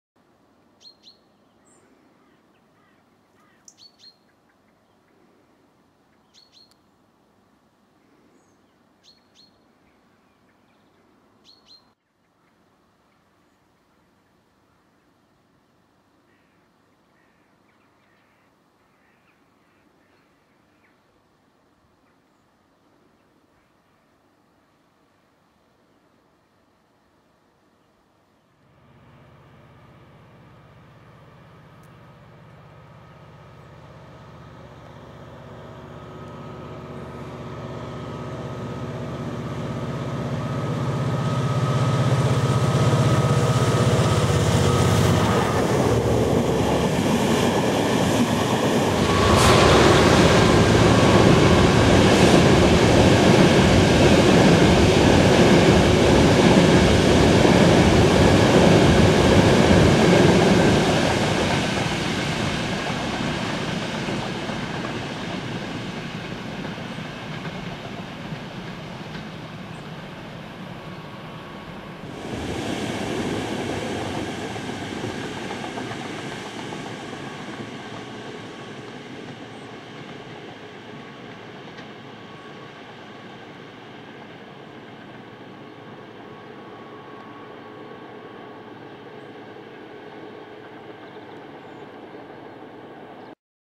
67021 +Class 91 passing Burton Pedwardine - 12/11/11
Class 67 67021 drags a Southbound diverted EC Class 91 passing Mount Lane Burton Pedwardine - 12/11/11